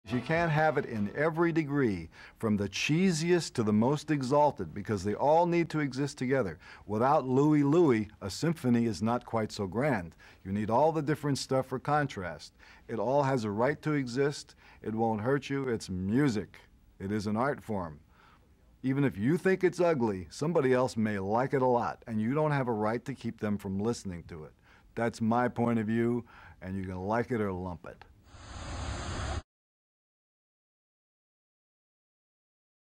音楽に対する自身の思想を話すフランク・ザッパ［翻訳ミュージシャン］
主にミュージシャンのインタビューの翻訳をアップロードしています！